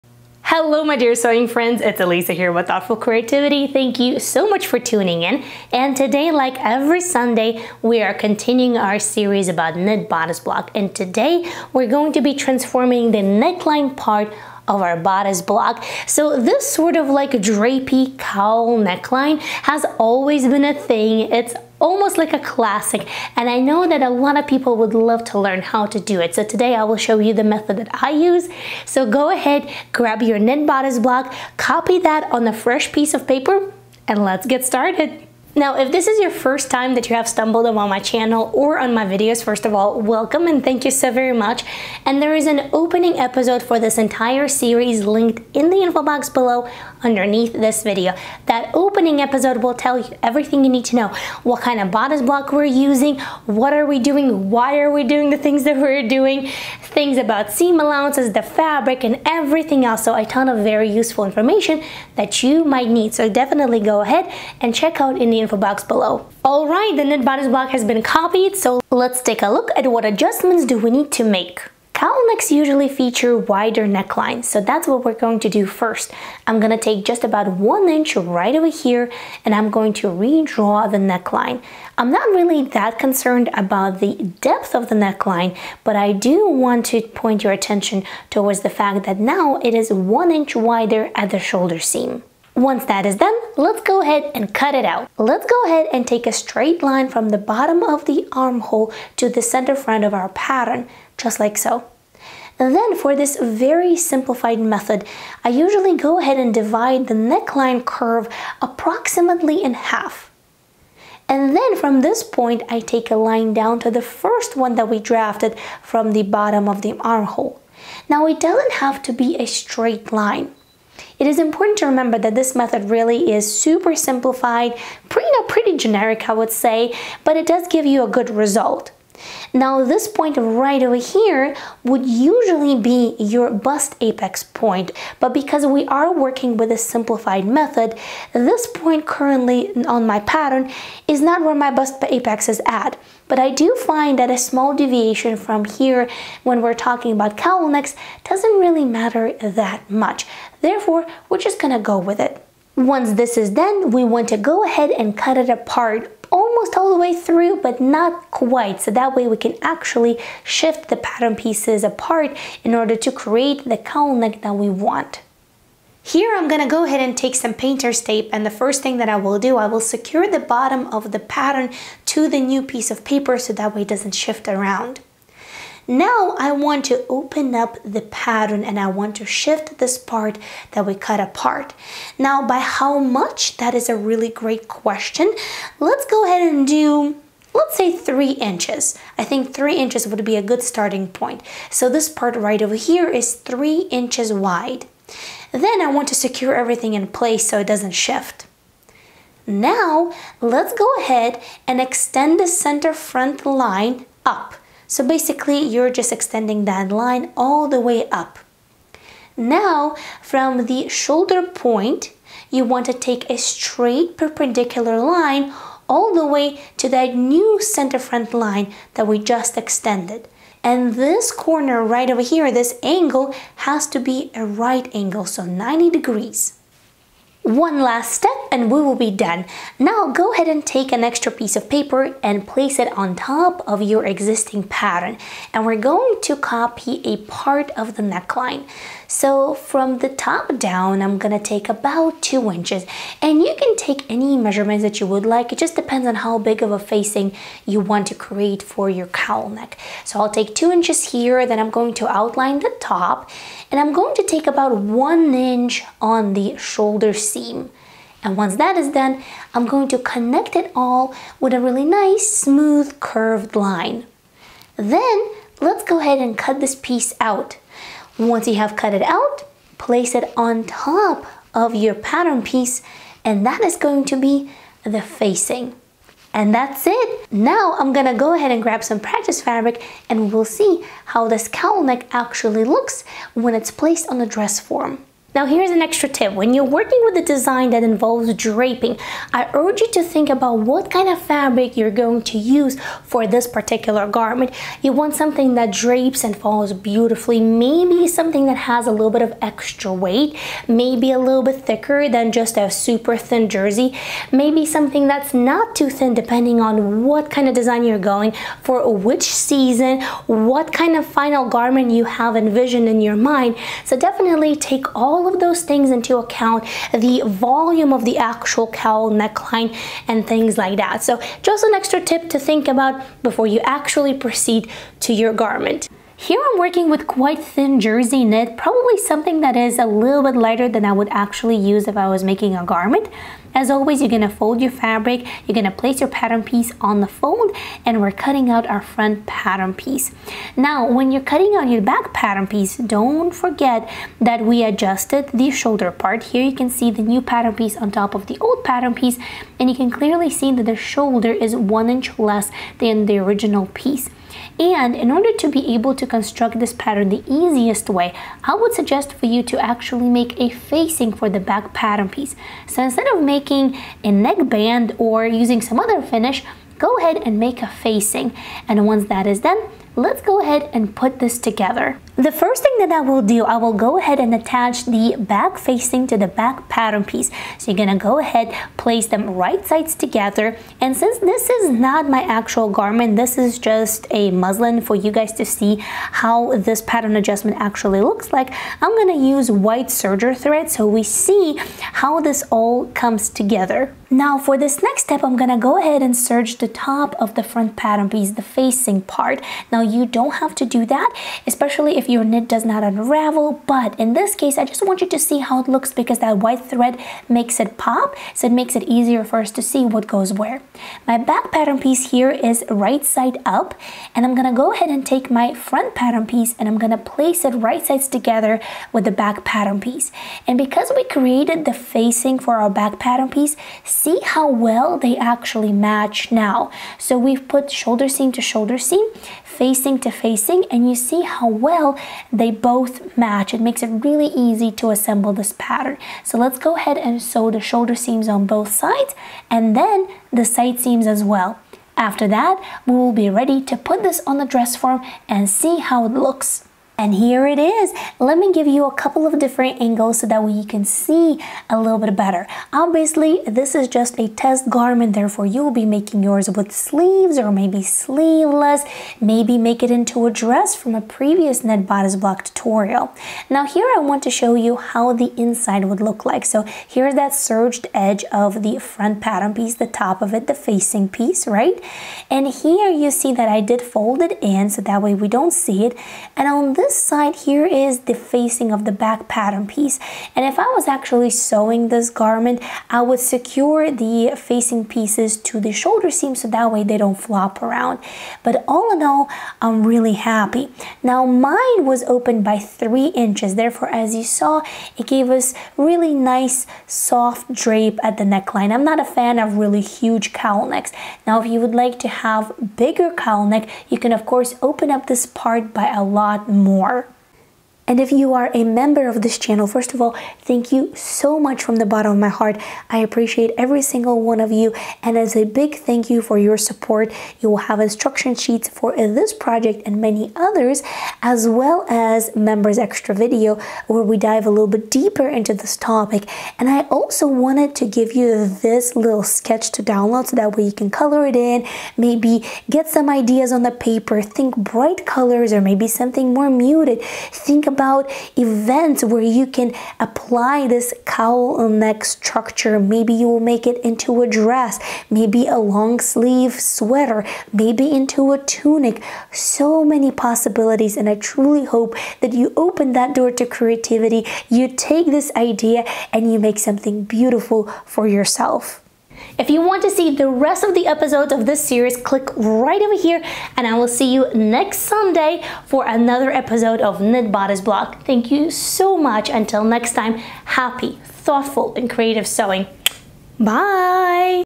How to draft a COWL neck from knit bodice block? (Ep 3)
3/8" = approx 0.95 cm
1/4" = approx 0.63 cm
1/2" = approx 1.27 cm
1" = approx 2.54 cm

Please remember - I am sharing with you my methods and my experience - you can use other methods or whatever gives you the best result. Find what works for you as there are MANY ways how to achieve success in sewing. Happy thoughtful and creative sewing!